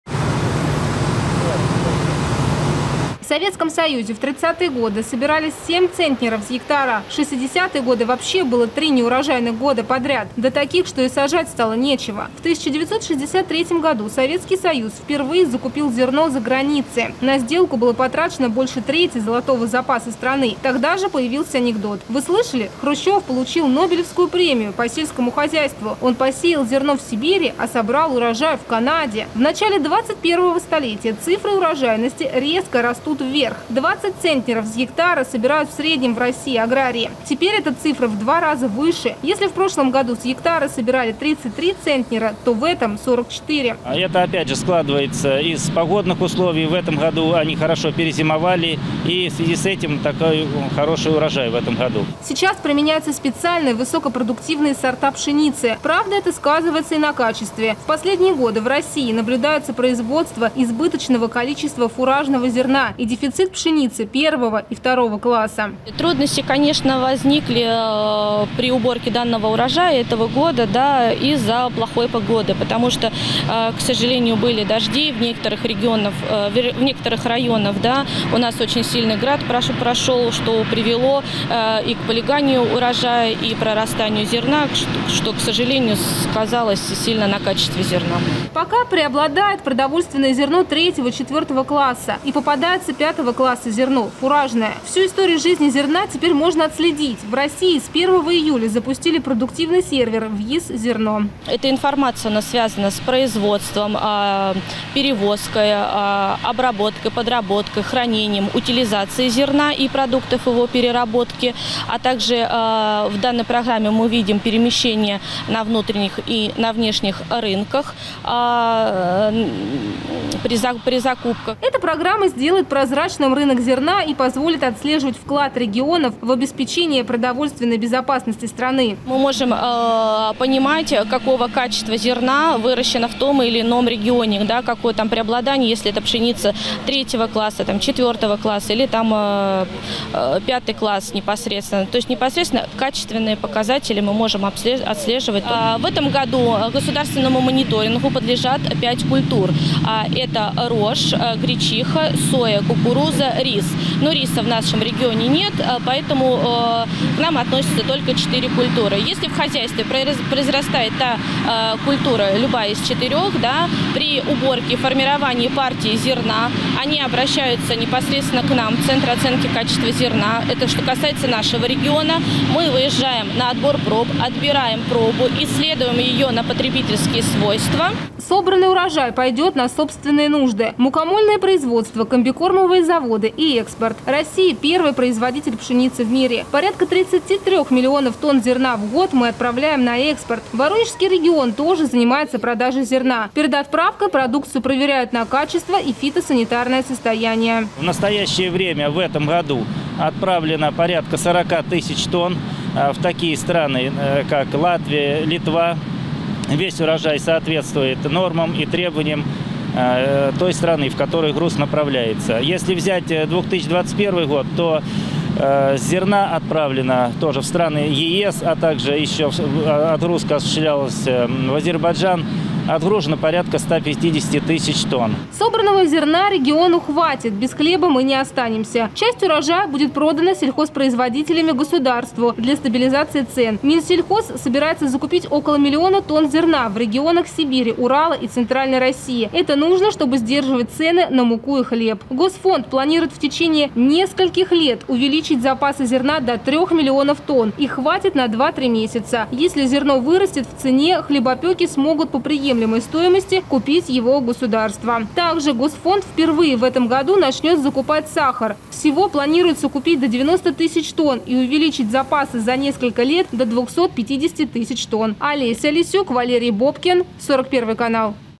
Как идёт уборка зерна?
Дождь и позднее начало уборочной компании – не помеха. В Воронежской области аграрии уже успели собрать второй миллион тонн зерна. Ожидается, что всего будет собрано 5 миллионов тонн зерна, это покроет нужды региона и позволит отправить часть урожая на экспорт. Об особенностях сбора урожая этого года смотрите наш следующий репортаж.